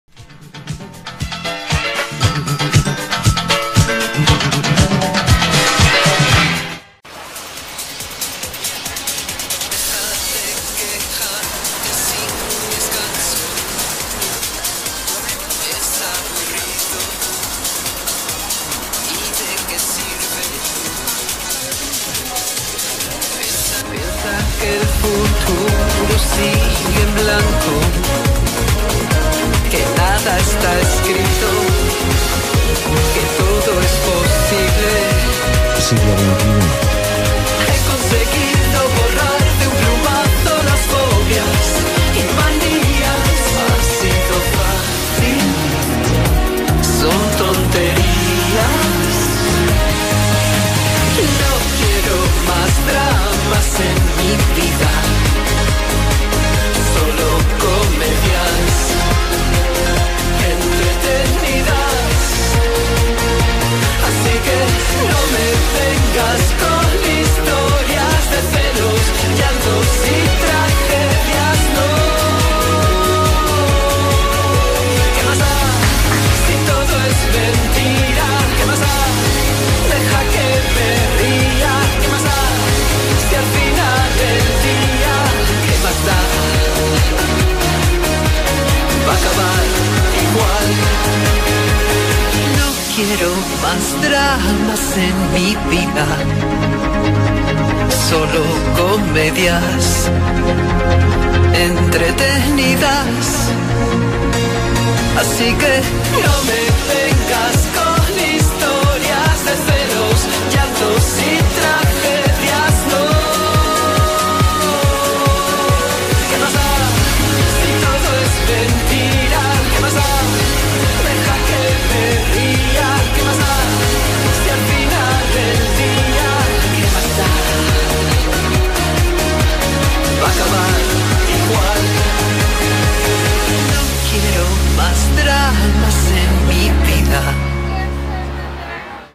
Alaska en el Brunch de Museo Chicote
Aquí os dejamos las imágenes del Brunch del Domingo en Museo Chicote. Tras la comida, Alaska nos mostró una sesión llena de clásicos españoles y americanos para bailar toda la tarde. En Museo Chicote, los domingos son otra cosa